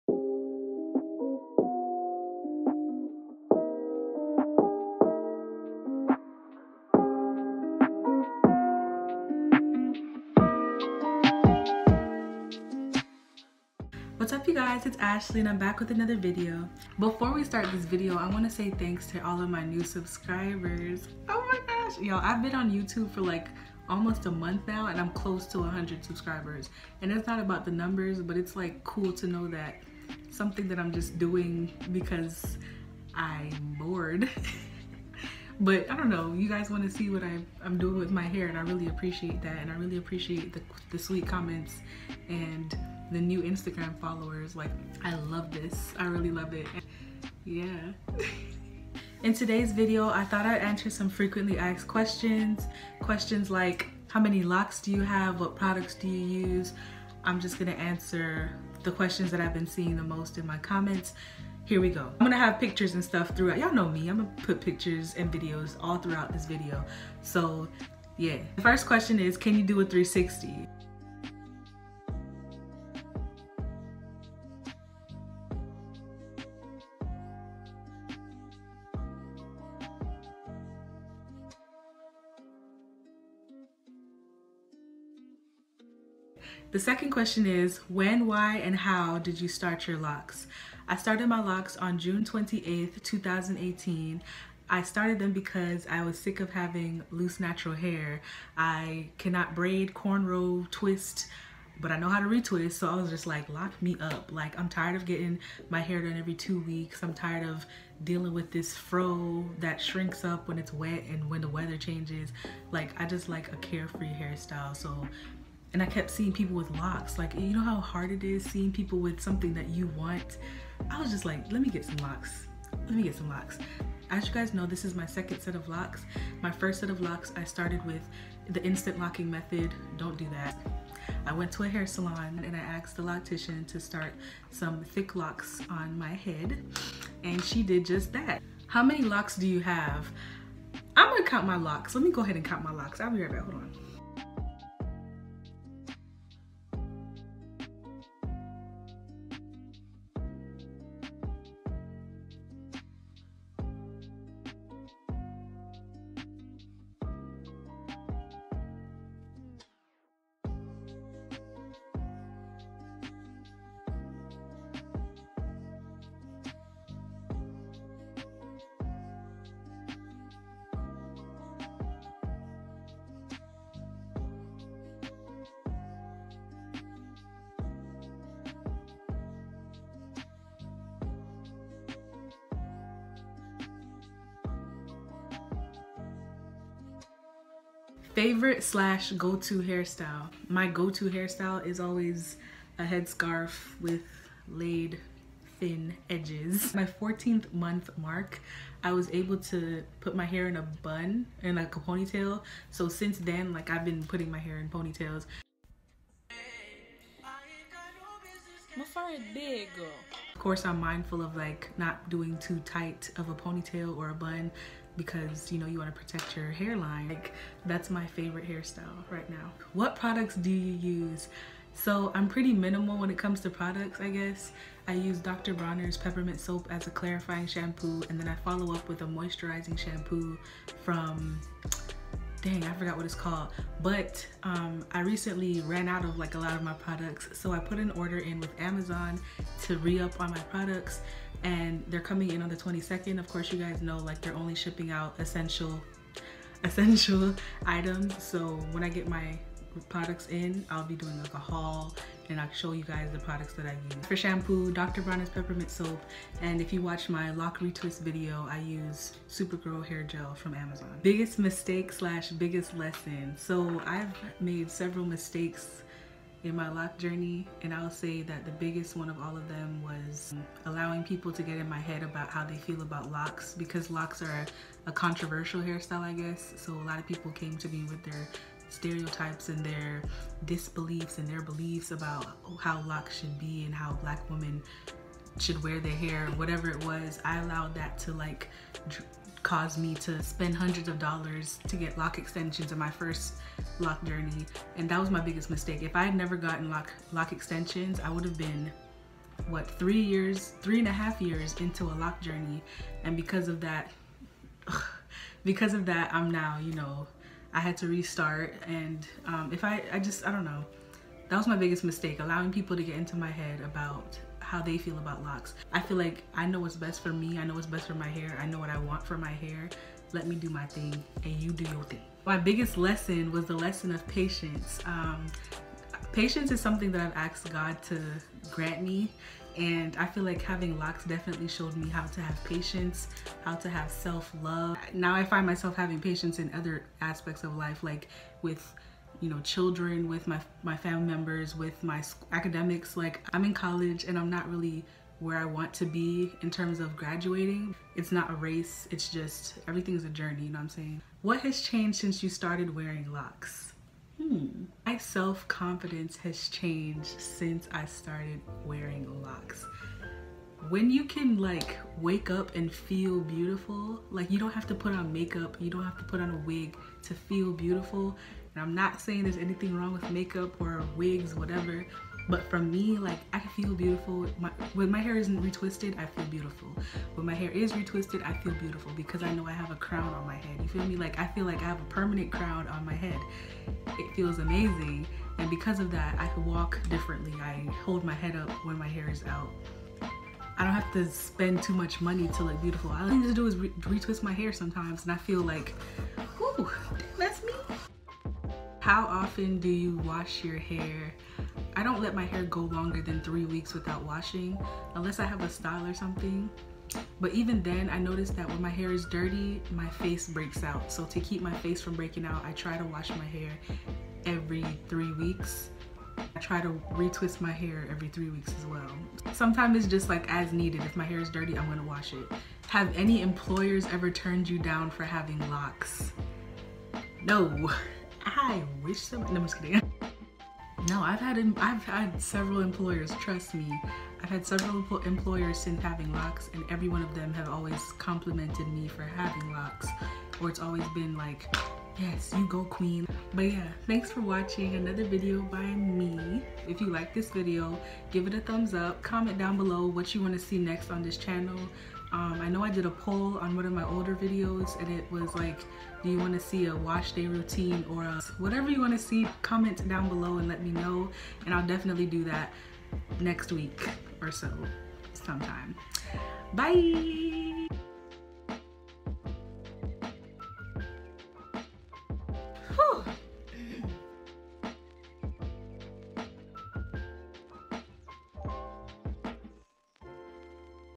FAQs | How Many LOCS Do I Have?!
Questions:
1. Do a 360
2. When, why, and how did you start your Locs?
3. How many Locs do you have?
4. Favorite/ go to hairstyle
5. What products do you use?
6. Biggest mistake/ biggest lesson?
7. What has changed since you started wearing locs?
8. How often do you wash your hair?
9. Have any employers ever turned you down for having Locs?